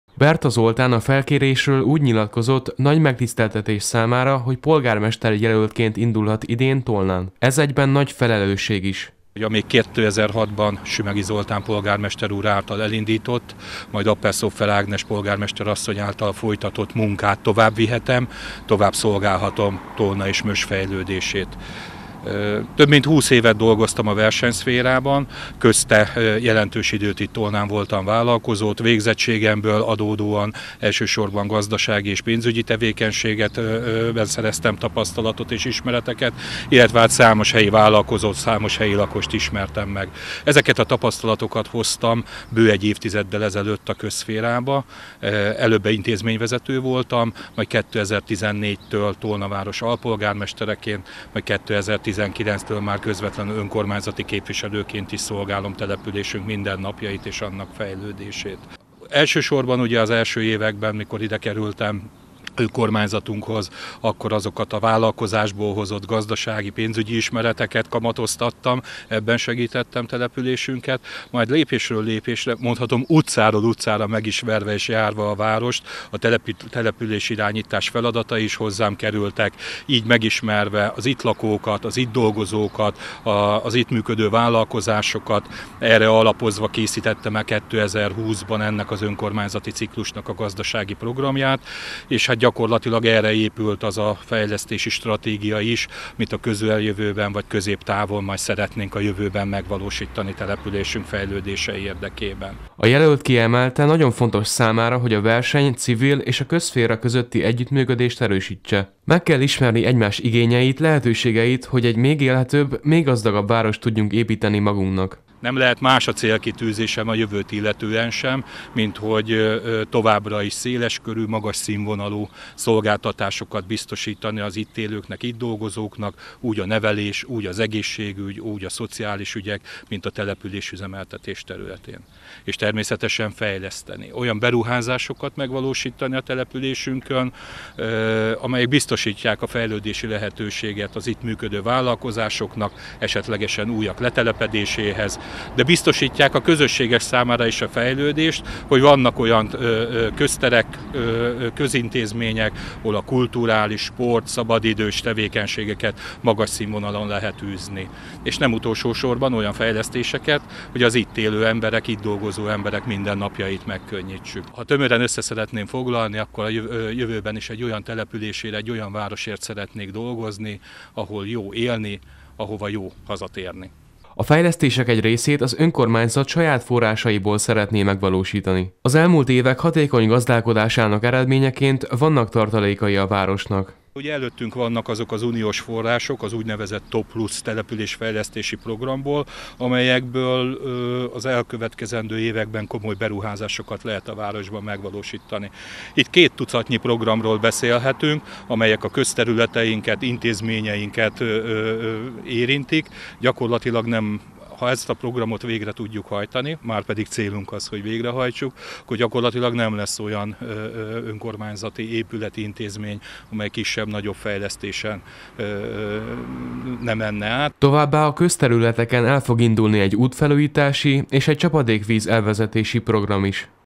Városvezetés - Berta Zoltán a Fidesz-KDNP polgármesterjelöltje Tolnán
Berta Zoltán lesz a FIDESZ-KDNP polgármesterjelöltje Tolnán, a júniusi önkormányzati választásokon. A politikus jelenleg az alpolgármesteri tisztséget tölti be a településen.